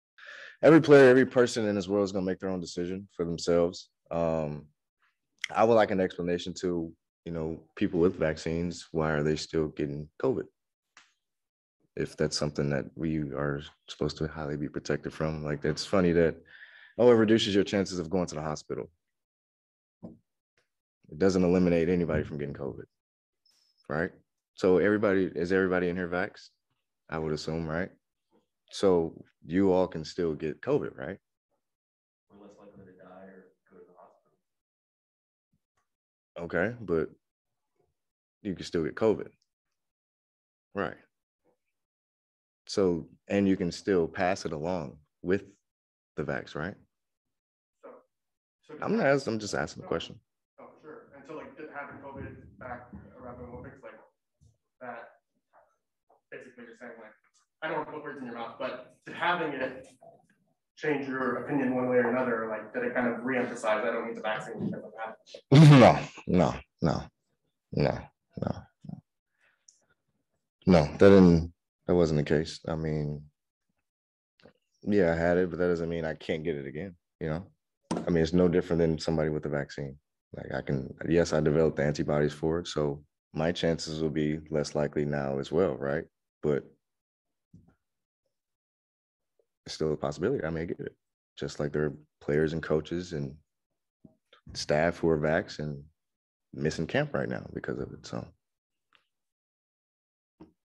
Bradley Beal. Why get the shot for COVID 19 Vaccine when I have Immunity and anti-bodies
Bradley Beal sounding off the badass alerts. Immunity is 27x more stronger than the vaccine.